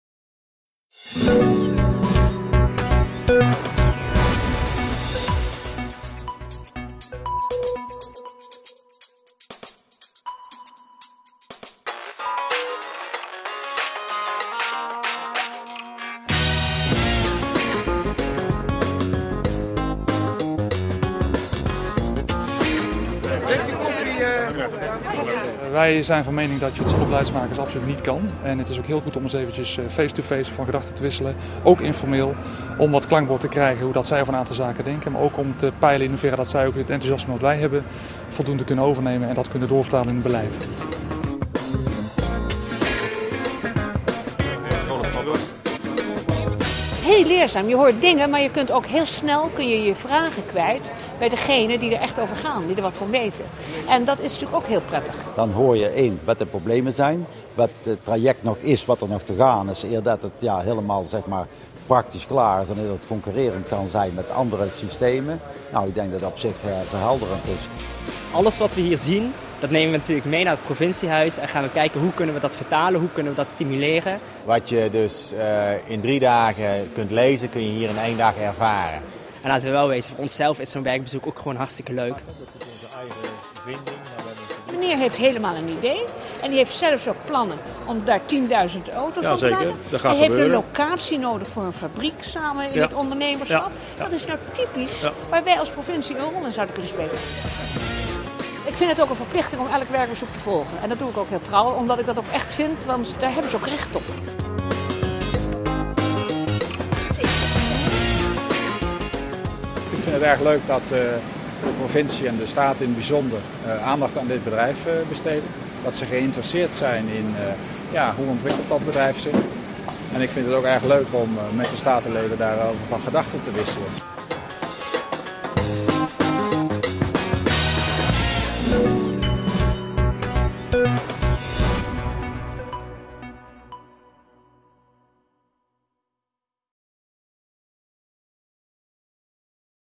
Statenleden op werkbezoek
Leden van Provinciale Staten gaan regelmatig op werkbezoek om zich te verdiepen en in de praktijk te laten bijpraten over de onderwerpen die op de politieke agenda staan.